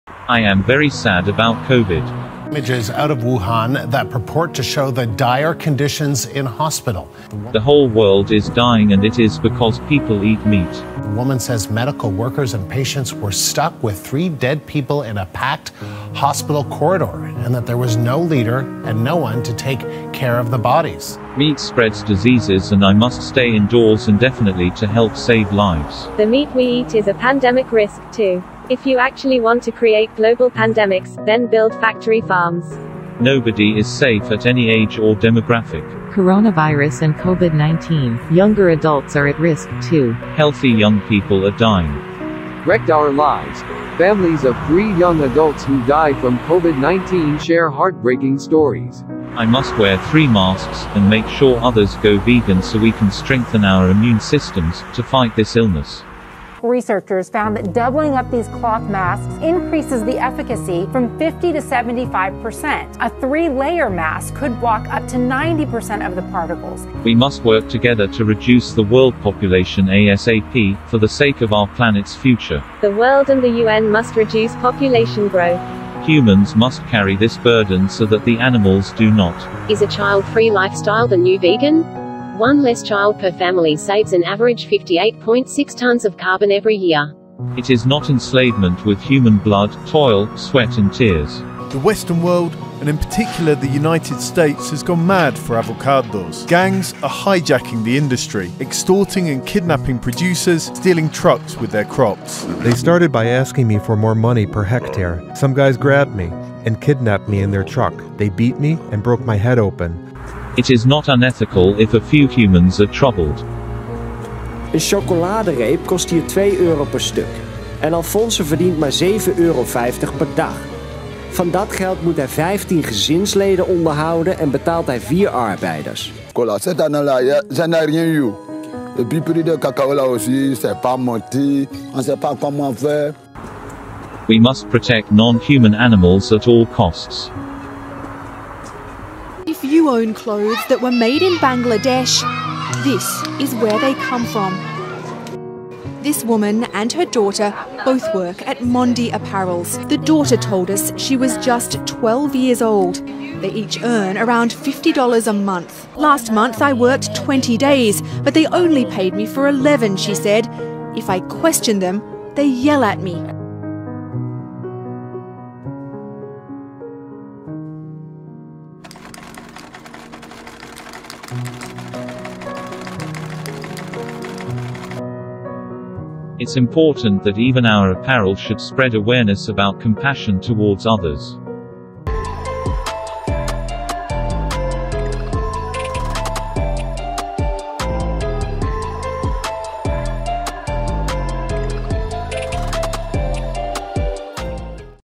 NPC Vegan talks Covid
tl;dw: NPC vegan talking about coronavirus and how being vegan helps prevent future covid pandemics from happening.